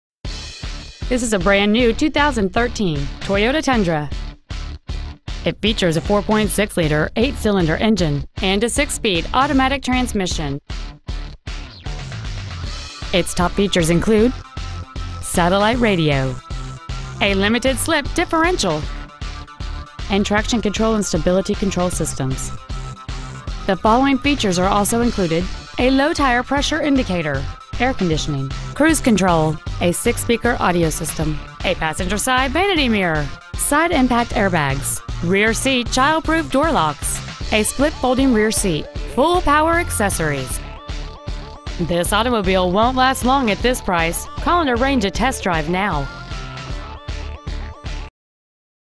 2013 Toyota Tundra 2WD Irving TX
We've been honored to serve the Irving TX area , we promise that your experience at our dealership will exceed your expectations ! Year : 2013 Make : Toyota Model : Tundra 2WD Engine : Gas V8 4.6L/285 Trans . : Automatic Exterior : Black Miles : 8 Toyota of Irving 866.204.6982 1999 W. Airport Frwy Irving , TX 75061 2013 Toyota Tundra 2WD Irving TX